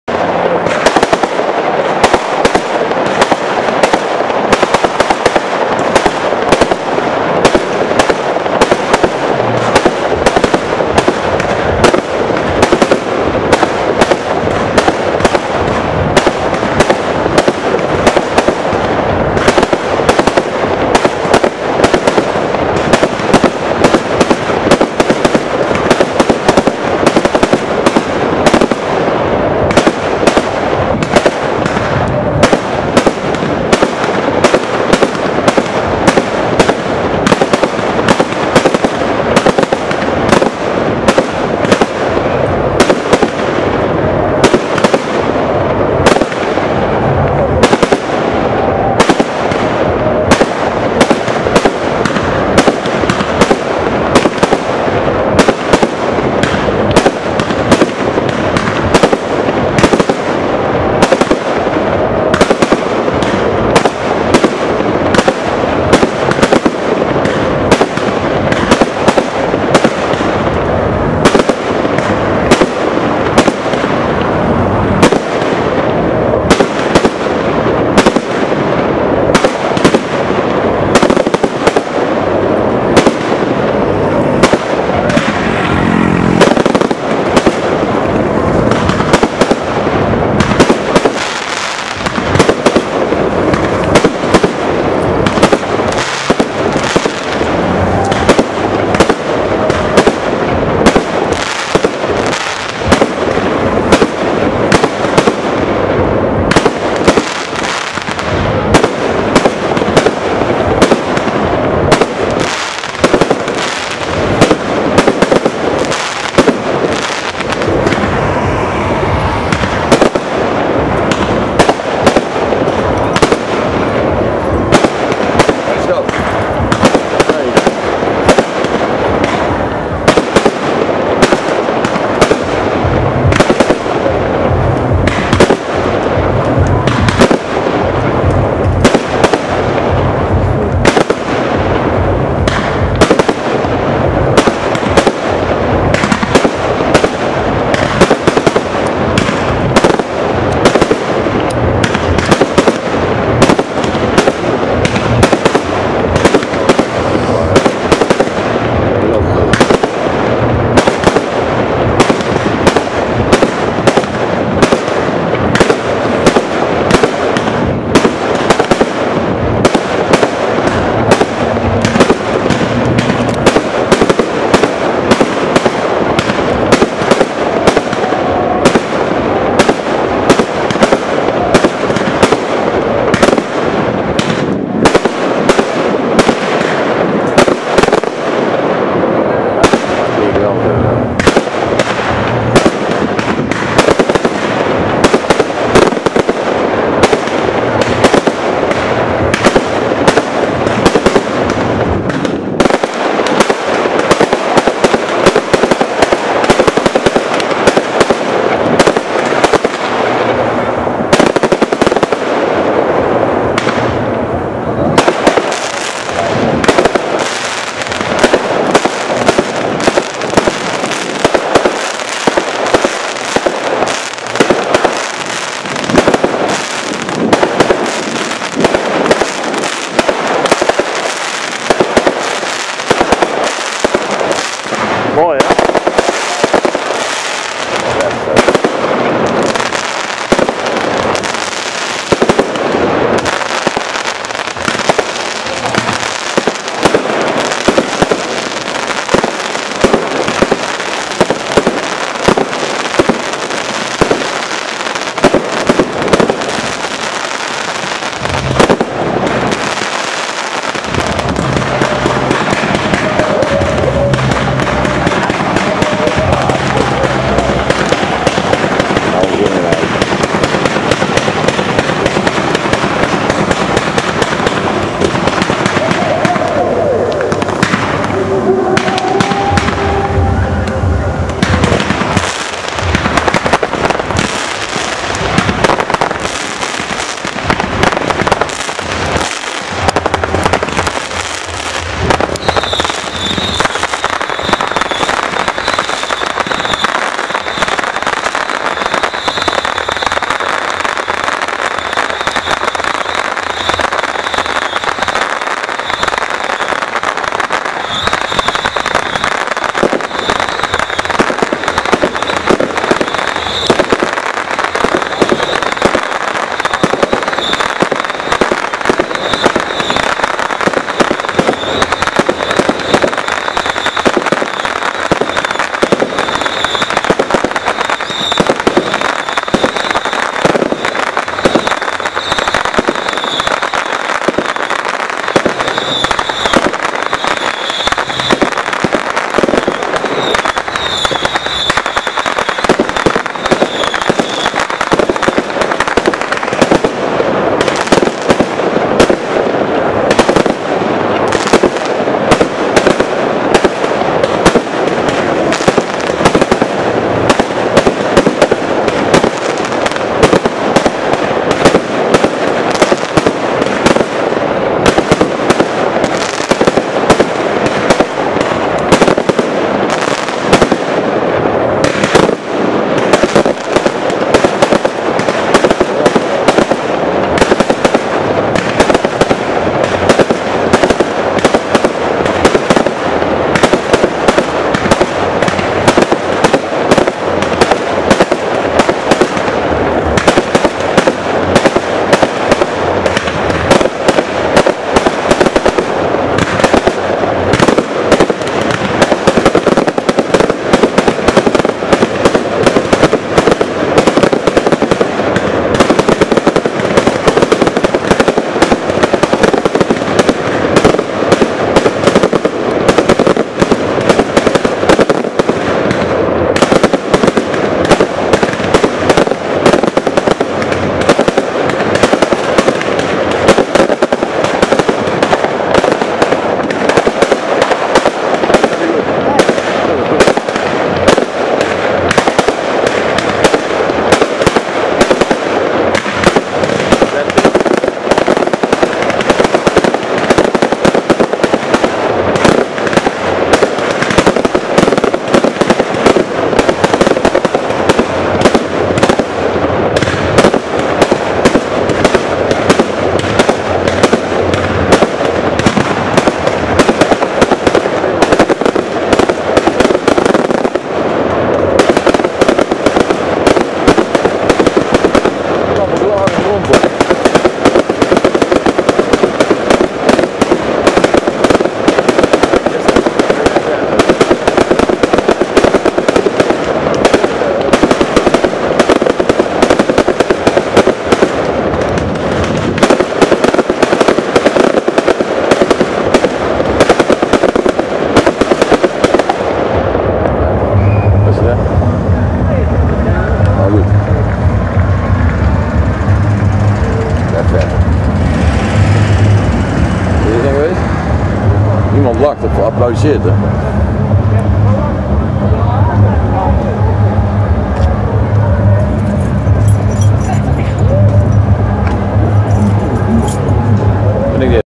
Vuurwerk kermis Schijndel 13-07-2011 Full HD
Een aardige vuurwerkshow van ruim 8 minuten op de kermis in schijndel.